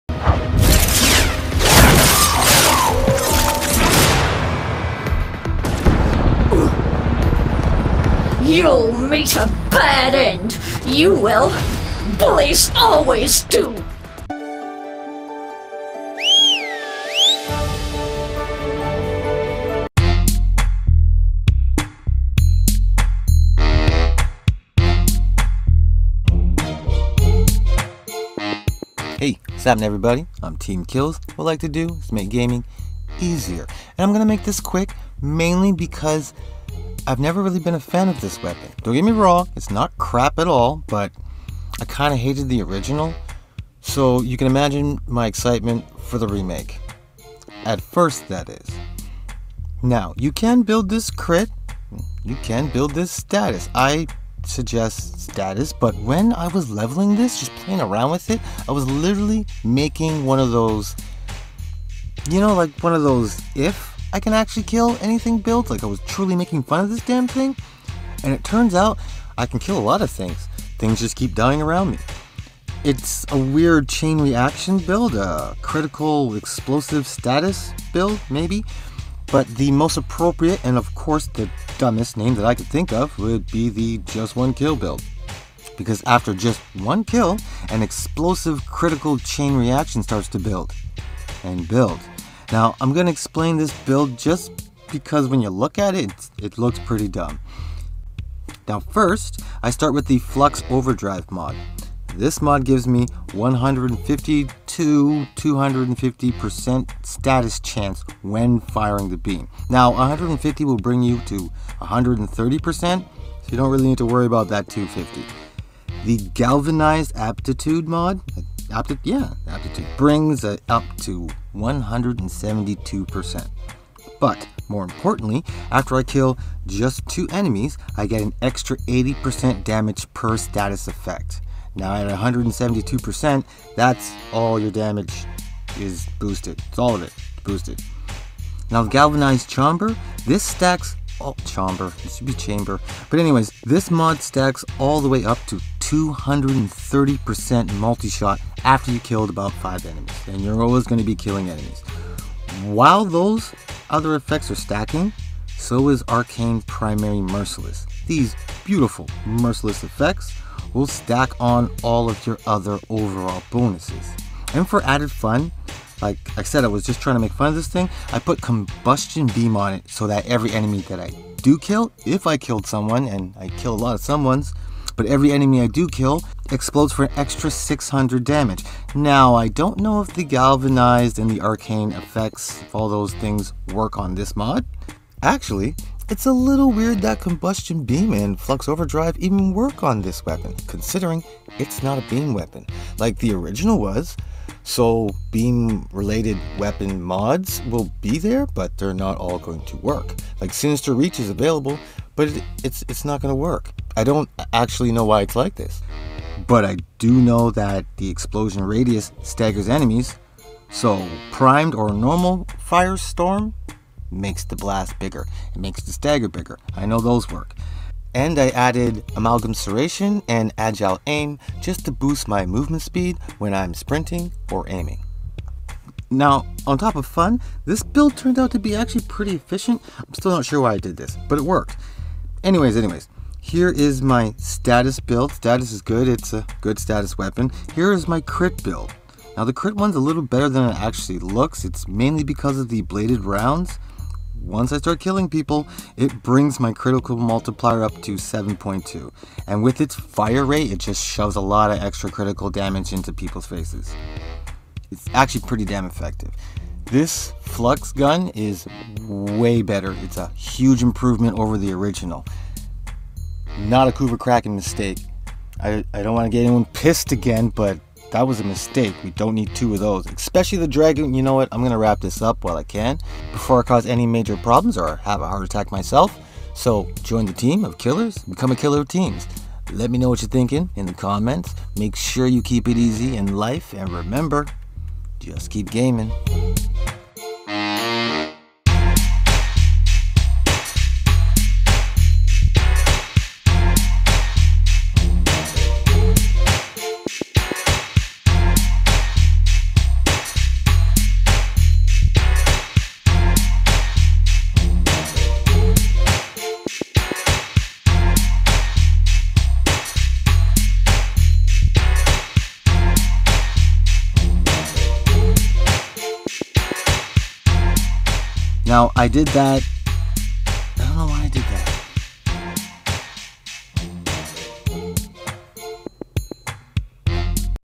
Tenet Flux Rifle Explosive Status Monster "Just 1 Kill Build"
Hello and welcome to my Tenet Flux Rifle "Just 1 Kill" Explosive Status Monster Video! This is one of the best surprises I've had in a long time. "I" do not like the Flux Rifle, but the Tenet Flux Rifle is a very different story! (for me) Check out the crazy good build I accidently stumbled across while try to make fun of this Tenet weapon. I'm beginning to believe in the Sisters! Join the team so you don't miss any other amazing discoveries, become a killer of teams! Make sure you keep it easy and remember, "Just Keep Gaming"